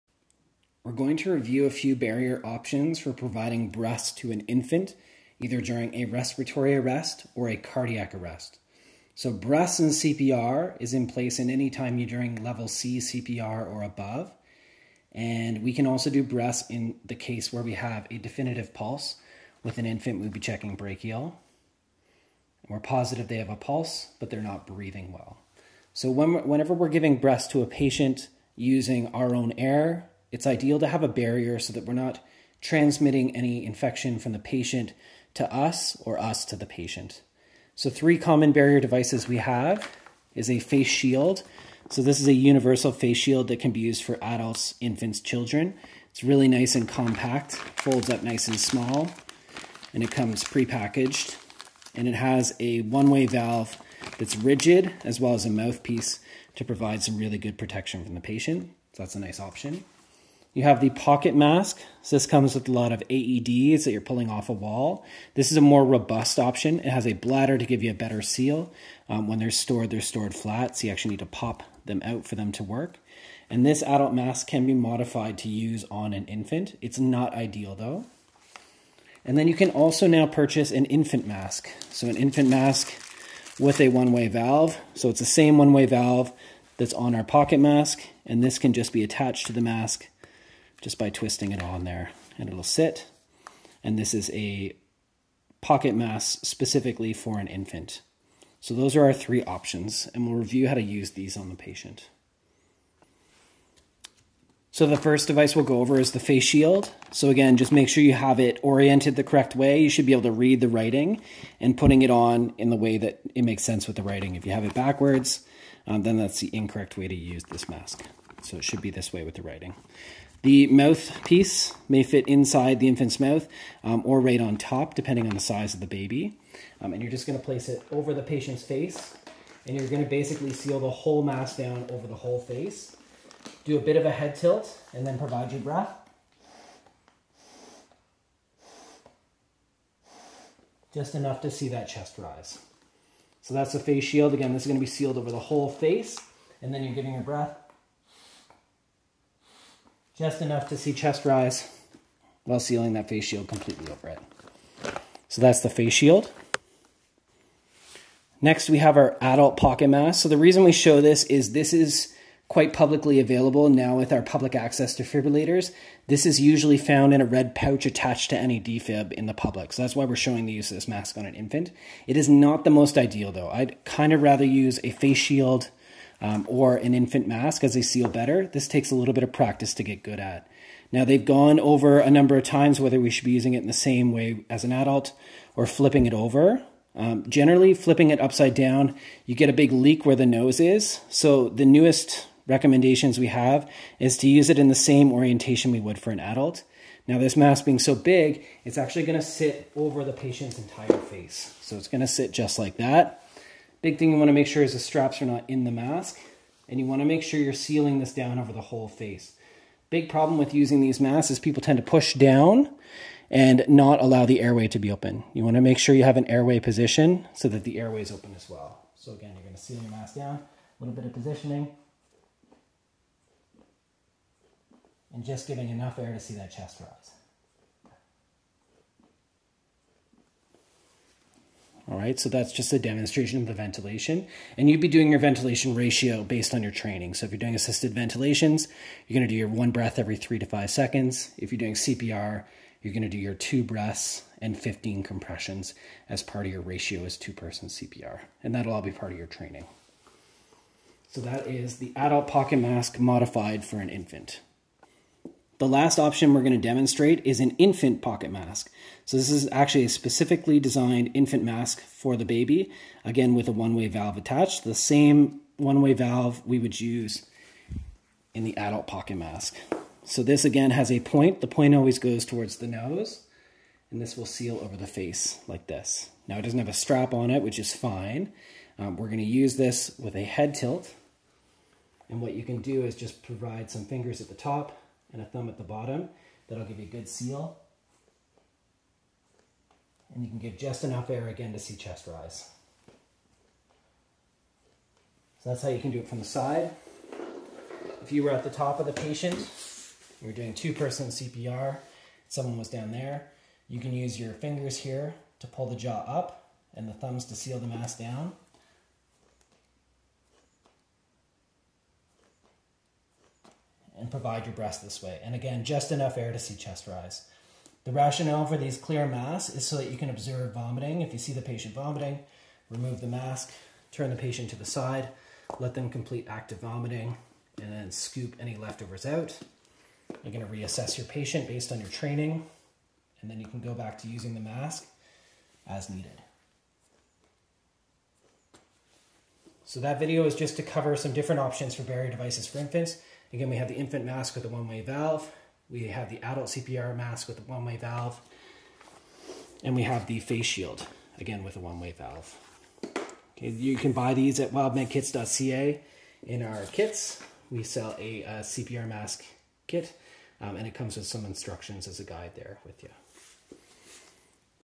Infant CPR Resuscitation Barrier Devices
This video reviews the use of barrier devices for providing assisted ventilations to infant patients.